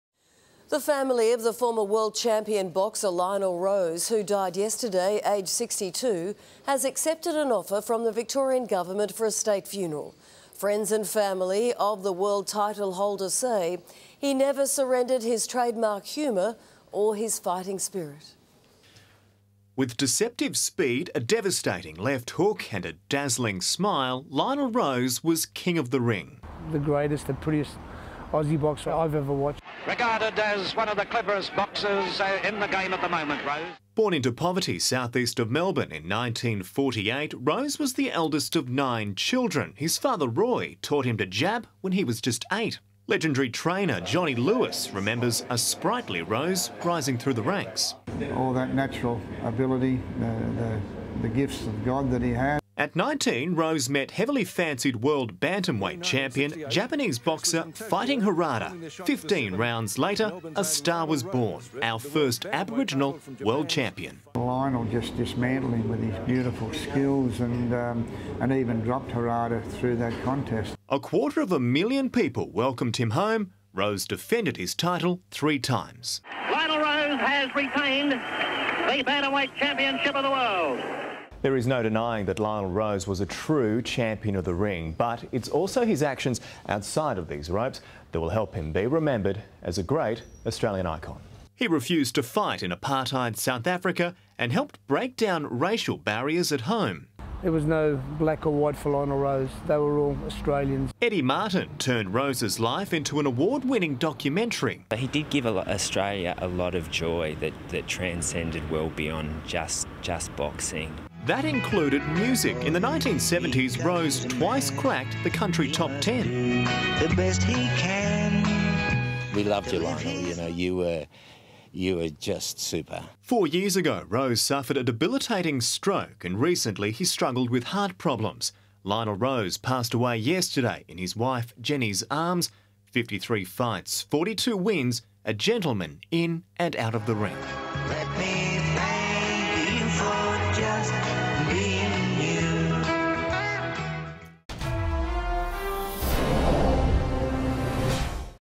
Lionel Rose's family accept Victorian government's offer
The family of the former world champion boxer Lionel Rose has accepted an offer from the Victorian government for a state funeral.